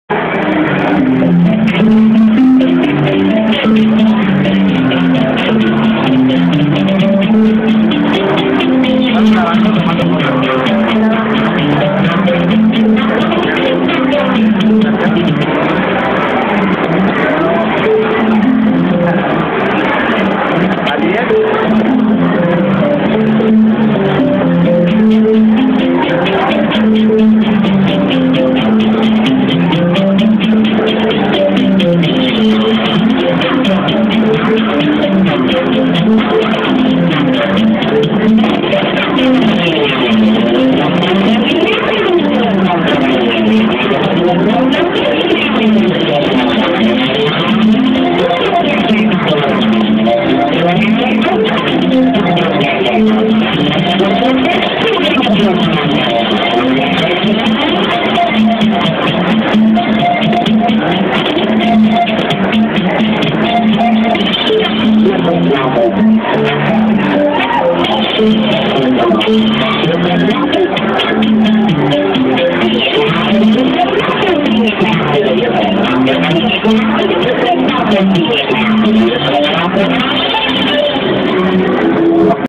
23022 Rafael Sanchez Terova
Esta es la rola de Rafael Sanchez Terova que grabo en la ITESM Puebla para participar en la promoción de Beat Edition.

¡¡¡Mucha Suerte!!!